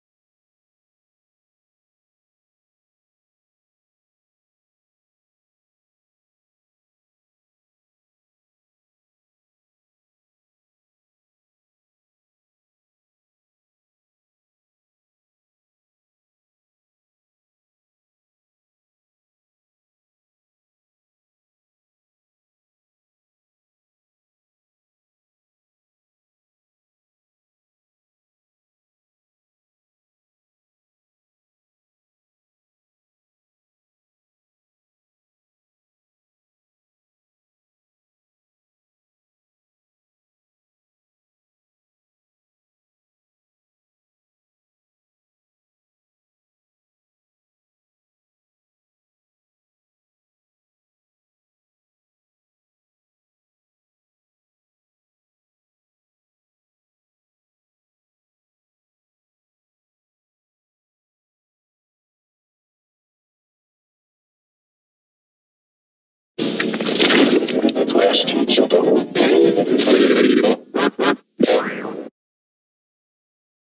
For Vinyl Scratch TheRussianVideoEditor (How to do Chocolate Milk on Sony Vegas)
Here's a tutorial on how to do Chocolate Milk on Sony Vegas